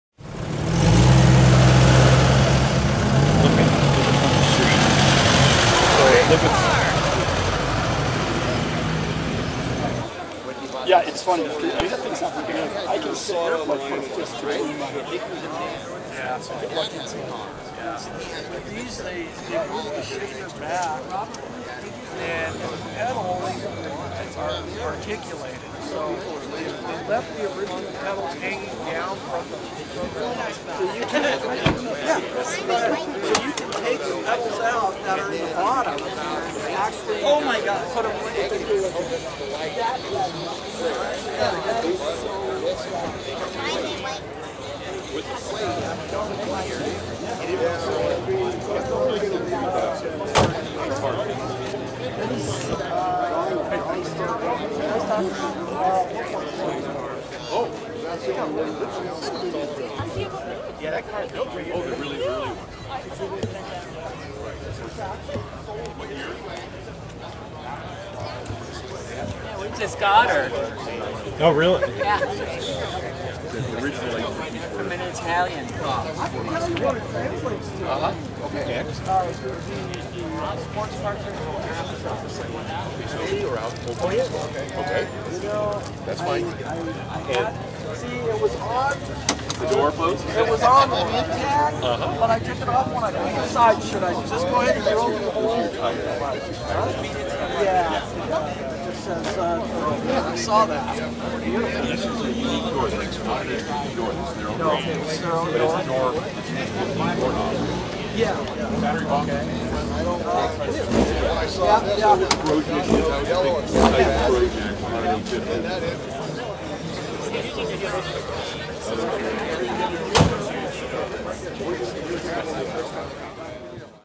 Alfa Romeo GTV
Footage from the Portland Cars & Coffee weekly meet, located at Langer's Entertainment Center in Sherwood, OR, of Alfa Romeo's (rare in U.S.) GTV.  Footage is off it coming into the meet and then some shots of it parked.  Enjoy!!  Shot 7/23/22